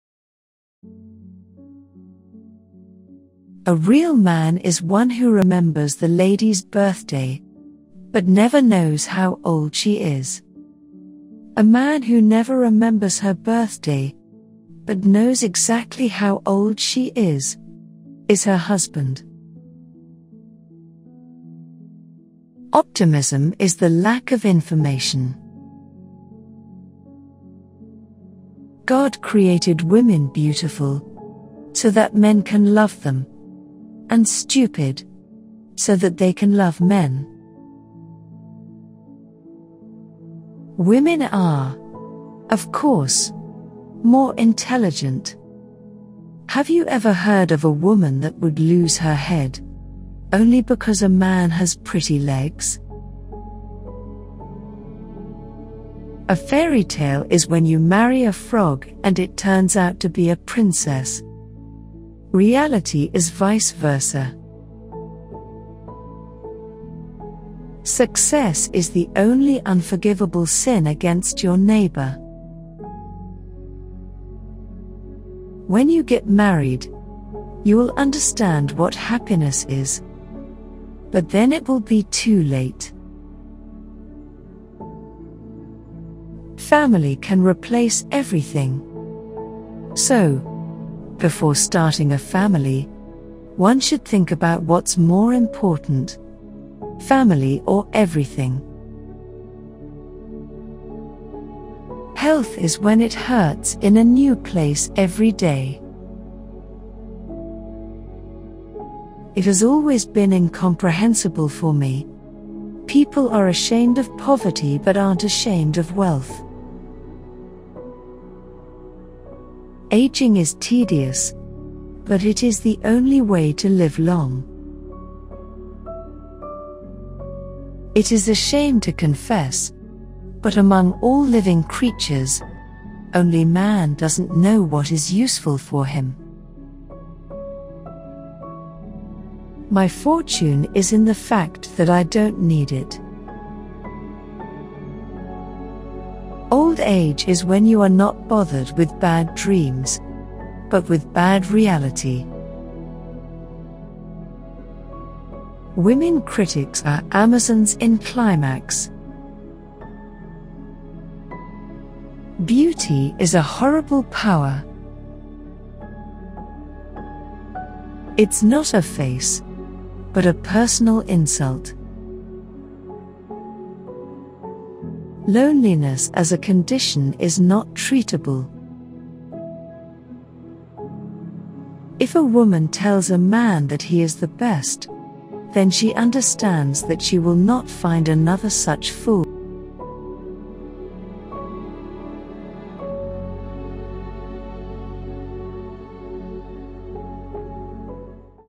Faina Ranevskaya - Wisest Quotes - How To Become Happy
Faina Georgievna Ranevskaya, is recognized as one of the greatest Soviet actresses in both tragedy and comedy. She was also famous for her aphorisms. She acted in plays by Anton Chekhov, Aleksandr Ostrovsky, Maxim Gorky, Ivan Krylov, Fyodor Dostoevsky, Leo Tolstoy, and others.

These are thoughts and quotes from Faina Ranevskaya. The best quotes and aphorisms of Faina Ranevskaya will make you think about many things in life. Best quotes from Faina Ranevskaya are definitely worth your time.

Your subscription may not seem like a big deal to you, but it makes all the difference to us.